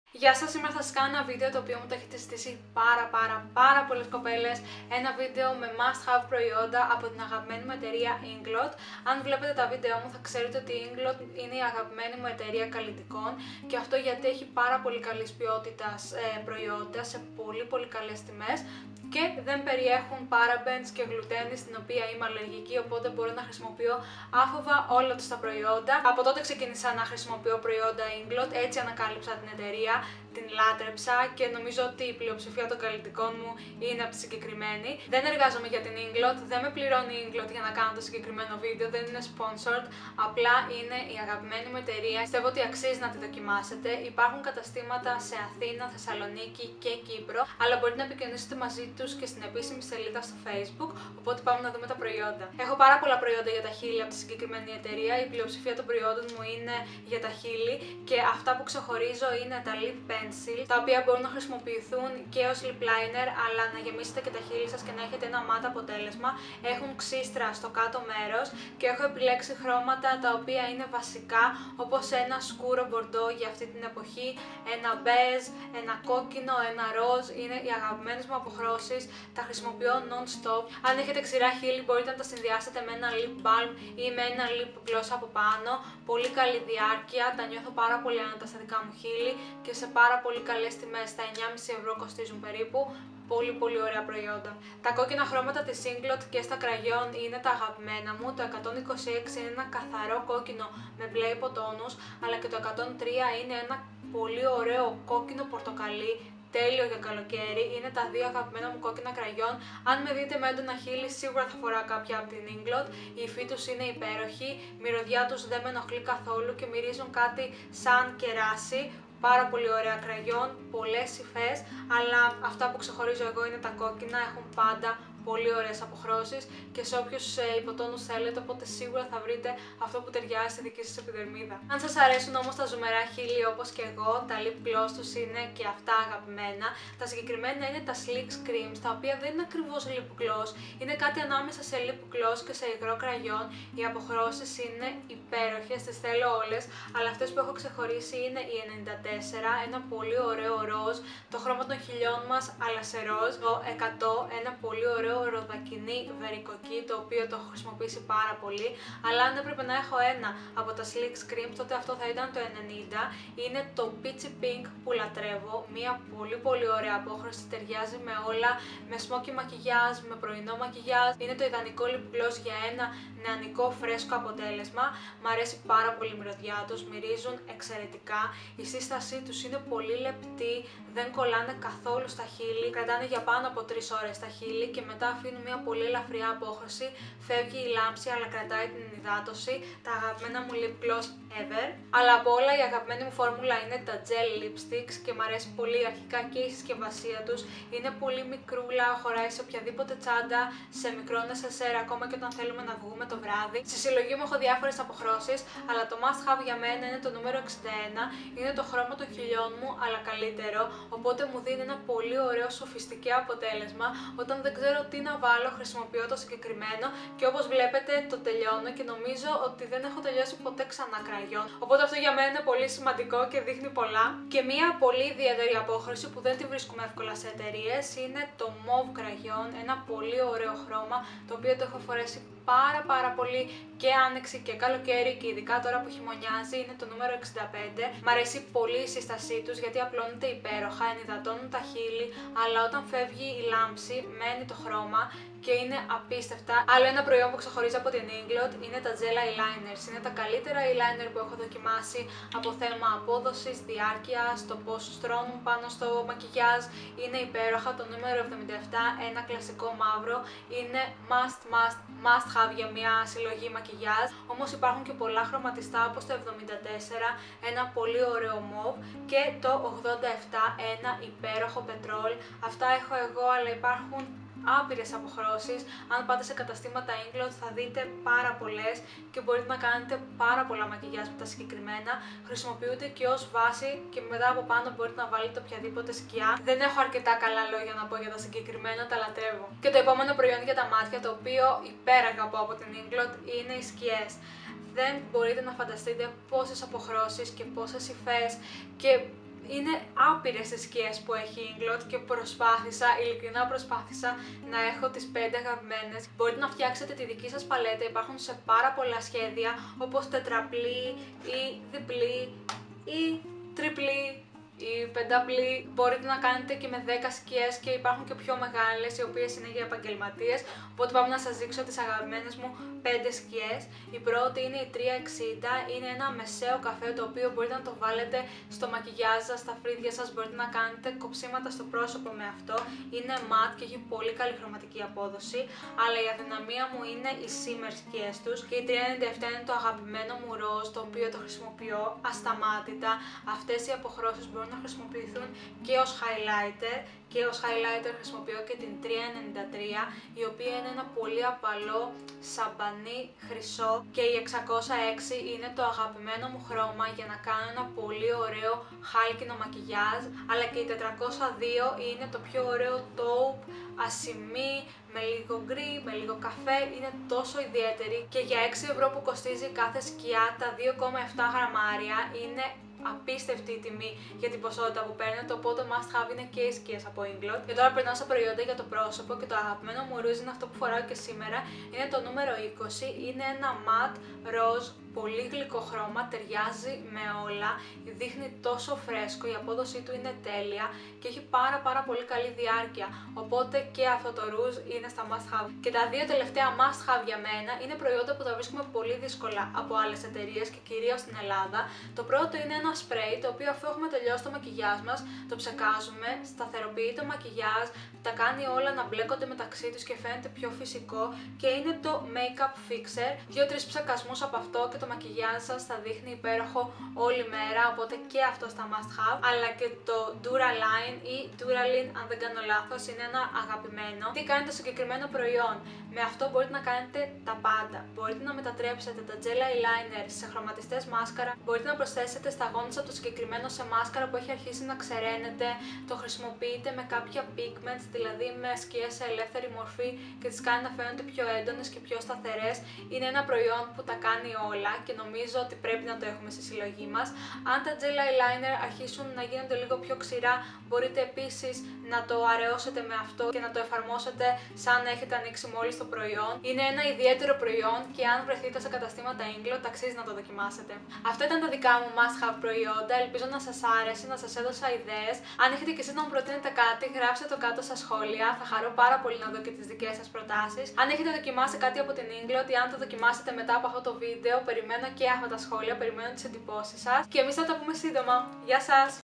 Lousousou Loves Inglot | My Must-have Products
VIDEO DETAILS

Και ναί εφτασε η στιγμή που θα σας μιλήσω για τα αγαπημένα μου Inglot προιόντα!
Καταστήματα υπάρχουν σε Αθήνα,Θεσσαλονίκη και Κύπρο αλλά μπορείτε να επικοινωνήσετε στην επίσημη σελίδα τους για παραπάνω πληροφορίες!
Love Lou!
PRODUCTS

AMC lip pencils : Pink no25,Red no21,Nude no17,Plum no33
Lipsticks : Red no126,Orange-red no103
Sleeks Cream : Pink no94,Peach no100,Peachy-Pink no90
Gel lipsticks : Pink no61,Peach no41,Nude no50,Lilac no65,Cherry no58
Gel eyeliners : Black no77,Plum no74,Teal no87
Eyeshadows : Brown no360,Pink no397,Champagne no393,Bronze no606,Taupe no402
Blush no20
Makeup Fixer
Duraline
I'M WEARING

Stradivarius Lace Shirt
Accessorize Earrings
Inglot Gel lipstick no58&AMC lip pencil no33
Use my gift code QVS877 at iHerb.com. You'll get $10 off your first order of $40 or more or $5 off orders less than $40!

FILMED AND EDITED BY LUCY KALAGKIA.